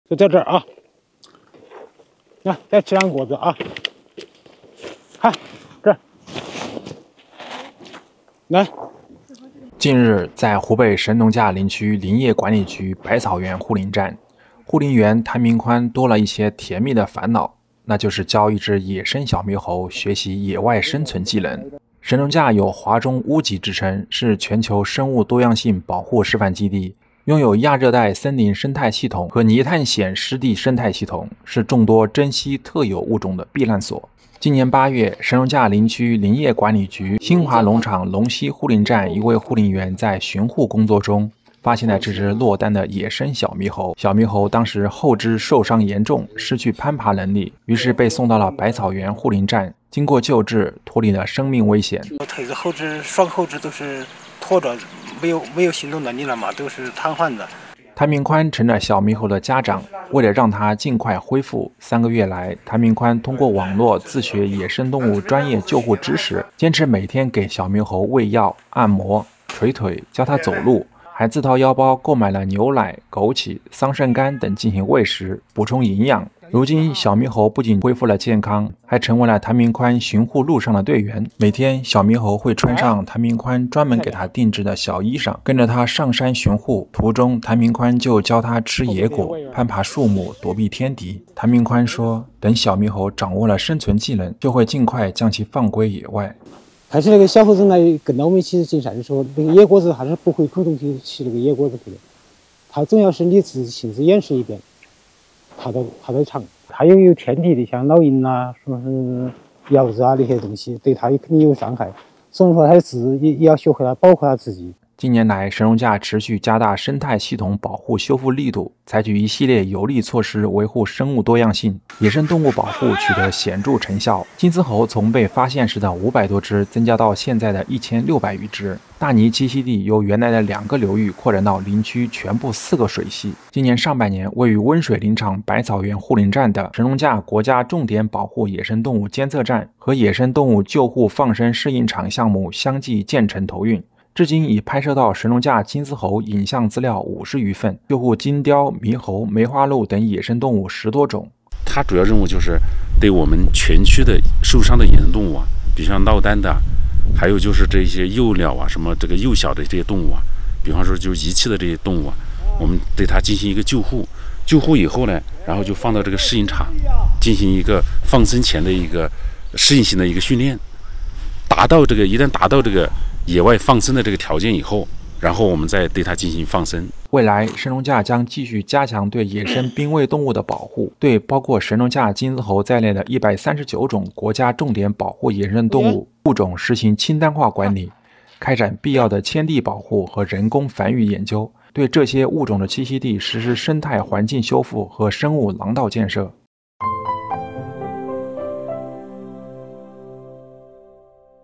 湖北神农架：护林员带着“野猴”去巡山
近日，在湖北神农架林区林业管理局百草园护林站，护林员谭明宽多了一些“甜蜜的烦恼”，那就是教一只野生小猕猴学习野外生存技能。三个月前，小猕猴受伤落单，神农架一位护林员将其带至百草园护林站进行救护。如今，小猕猴不仅恢复了健康，还成为了谭明宽巡护路上的“队员”。 等小猕猴掌握了生存技能后，谭明宽会将其放归野外。